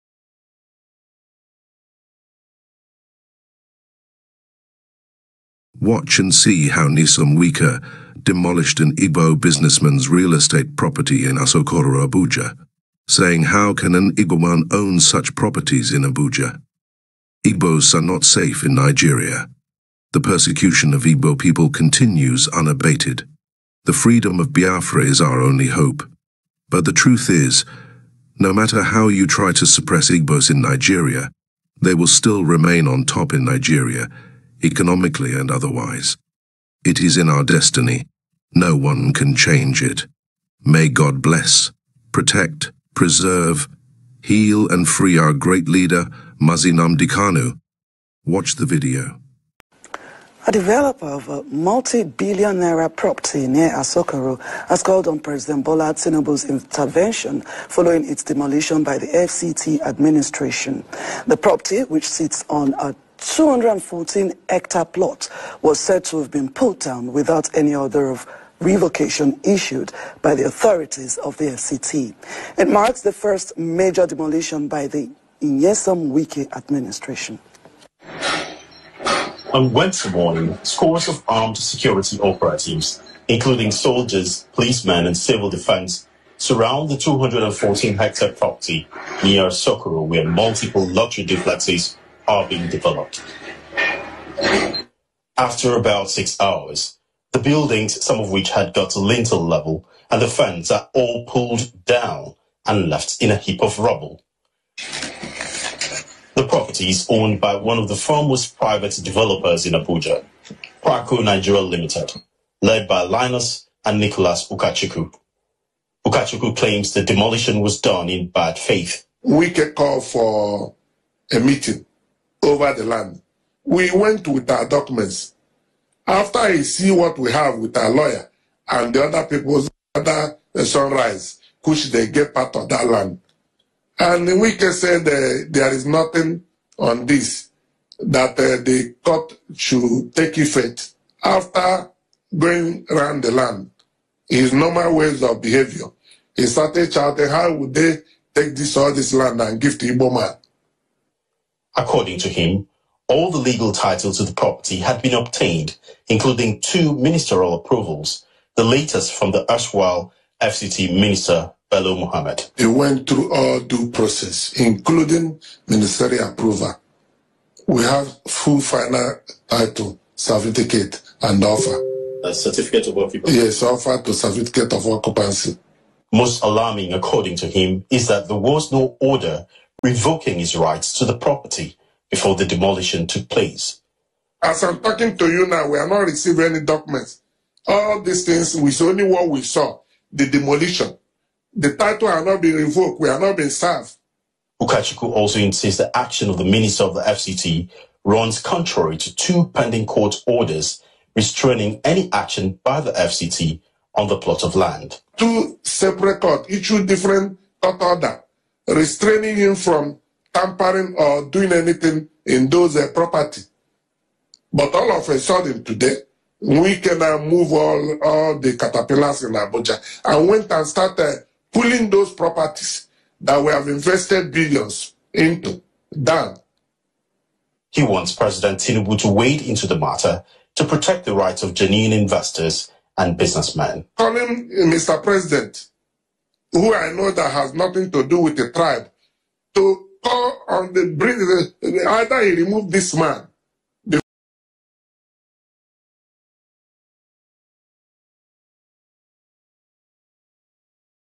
How can an Igbo man own such properties in Abuja ~ Nyesom Wike as he destroyed Igbo Man's Property.
How can an Igbo man own such properties in Abuja ~ Nyesom Wike says as he demolishes an Igbo Man's real estate property in abuja